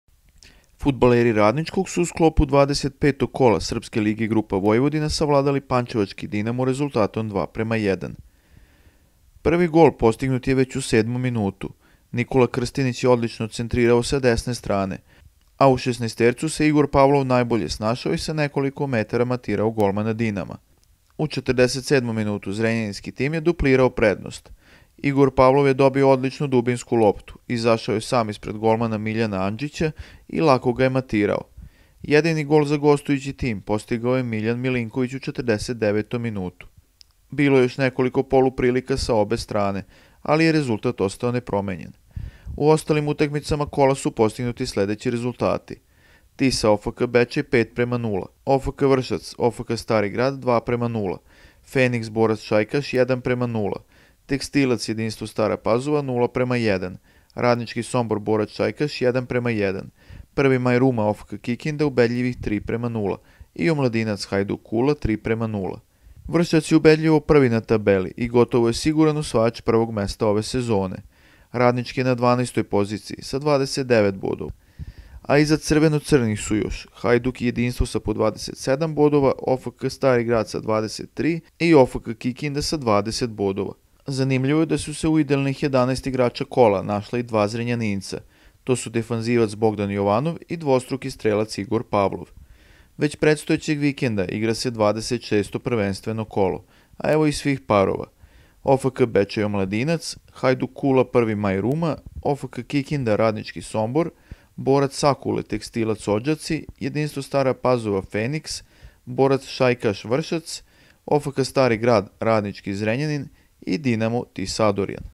RADNIČKI SVE BLIŽE OPSTANKU
Srpska liga grupa "Vojvodina" stigla je do 25. kola, a u poslednjoj prvenstvenoj utakmici fudbaleri Radničkog su savladali pančevački Dinamo i time se približili cilju, a to je opstanak u ovom rangu takmičenja.